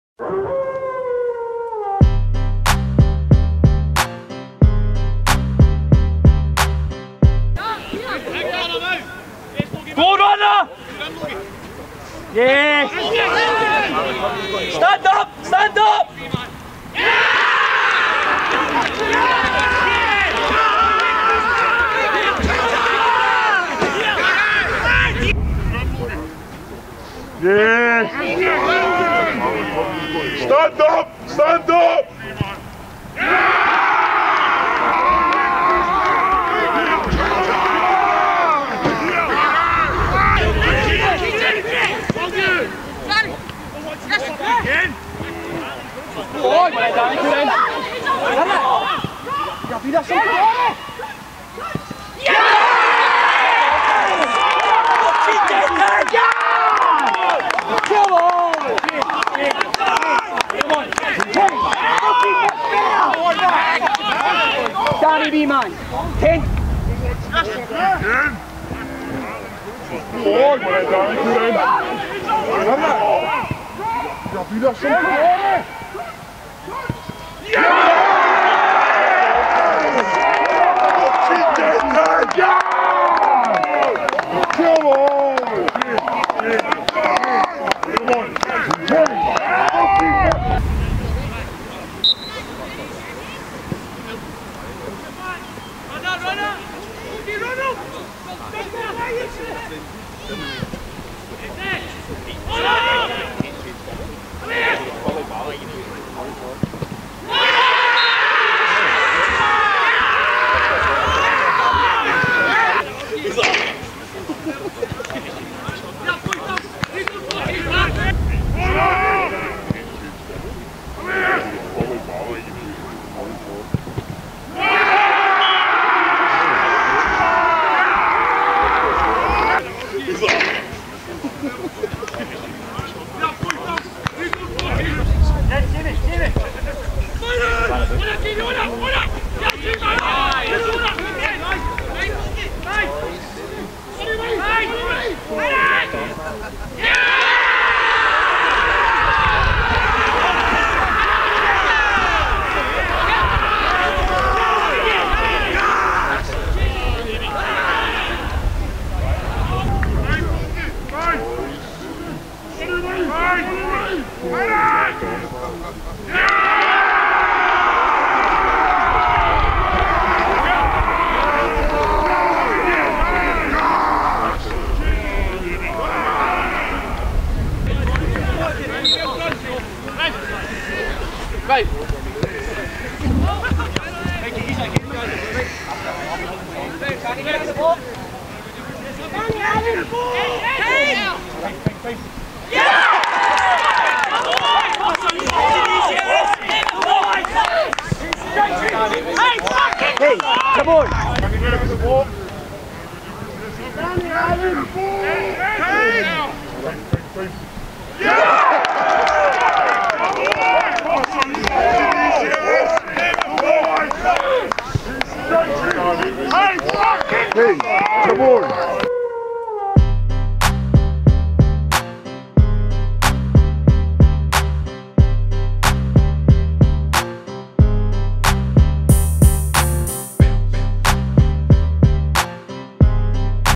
GOALS: Easterhouse 3-2 East Kilbride YM - SMAFA Division 1B - 15/04/23
Goals from this SMAFA Division 1B Fixture.

Easterhouse 3-2 East Kilbride YM

Goals for Easterhouse,
Jordan Logan x2 & Darren Hogan

Goals for East Kilbride,
Jordan Connor x2

All Footage Owned & Edited by Saltire Digital Media.